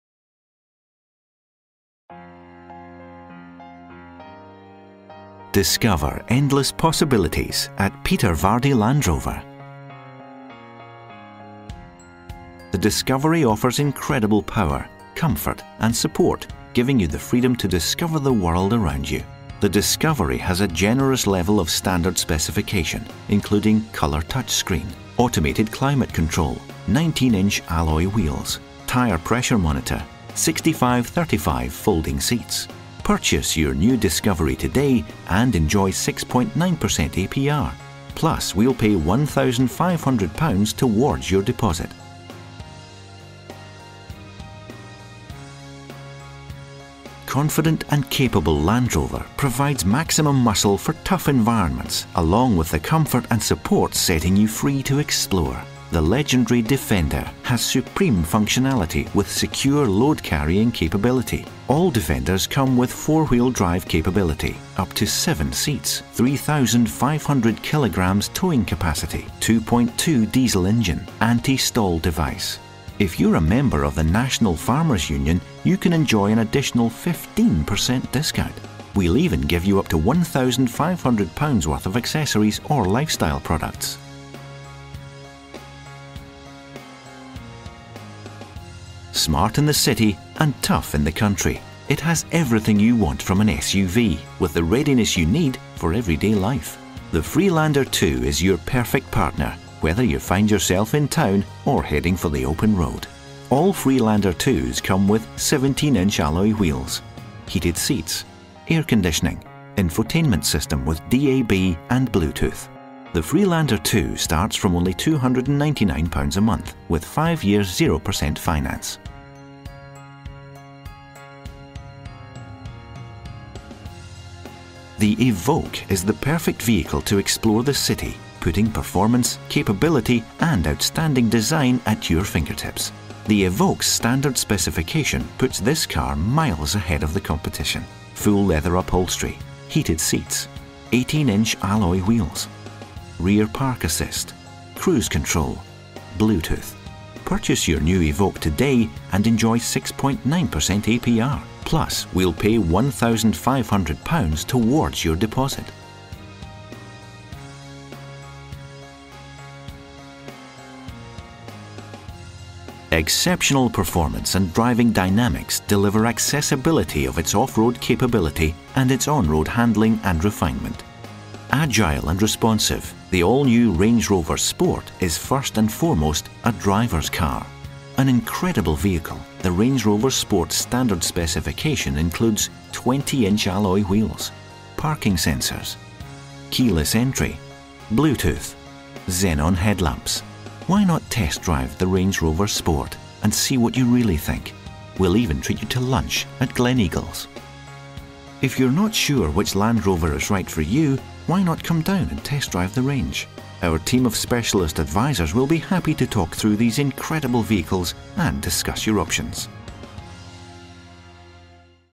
The Land Rover Range at Peter Vardy.
The Peter Vardy team at Land Rover in Aberdeen are pleased to be able to walk you through range available at our showroom in Aberdeen.